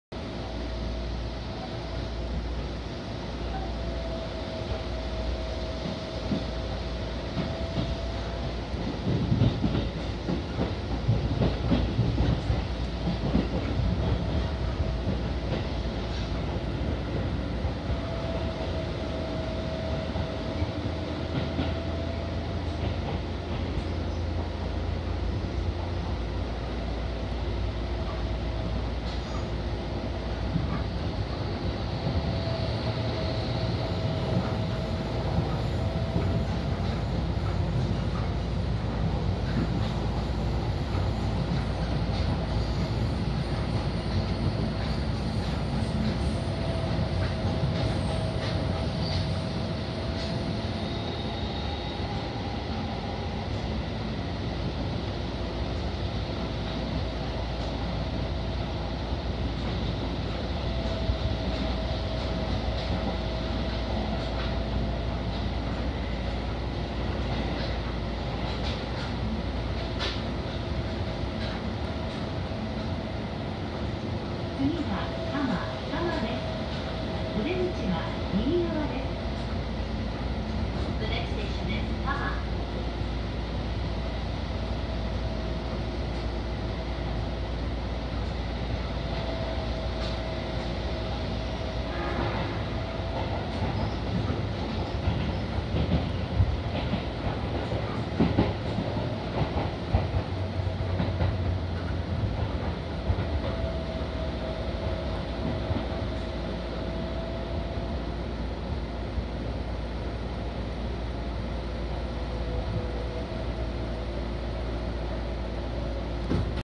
2019,9.7(土)14:02 西武多摩川線の門型鉄塔群と改良工事進む多磨駅【白糸台〜多摩 前面展望】
【西武多摩川線の門型鉄塔群】
整然と並ぶ高さ30-40mの門型鉄塔群は、昭和42年頃(1967年頃)に土地と工期を節約するために東京電力が線路上に設置したそうです。

【多磨駅】
多磨駅は、東口に出来る大規模商業施設と、東京オリンピックの会場周辺駅になることに伴う利用客増加への対応目的に改良工事中です。

従来は2面2線の対面式ホームの駅でしたが、上りホームを撤去して1線化した上でホームを拡幅、橋上駅舎化するとともに、東口を新設するそうです。
5月26日から使用が停止された上りホームは半分以上が解体され、上り線のレールも撤去済でした。
2020年の東京オリンピック前の供用開始を目指しているとのことで、完成が楽しみです。